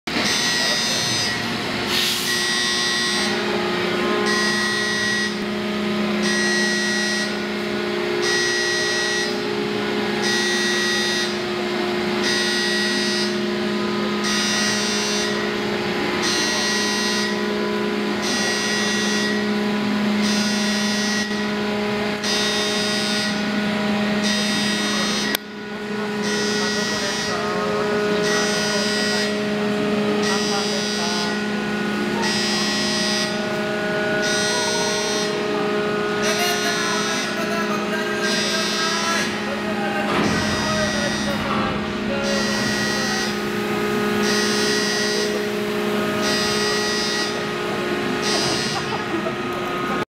護衛艦いせの後方エレベーター上昇
大阪南港で海自の護衛艦いせの一般公開があったので行ってきました

海自では金曜日にカレーが定番ですが、この日はどういうわけか土曜日にカレーだったため、船内からどこからともなくカレーのにおいが...

日本の安全のため誇りをもって仕事される自衛官に感謝！

2013年9月28日大阪南港にて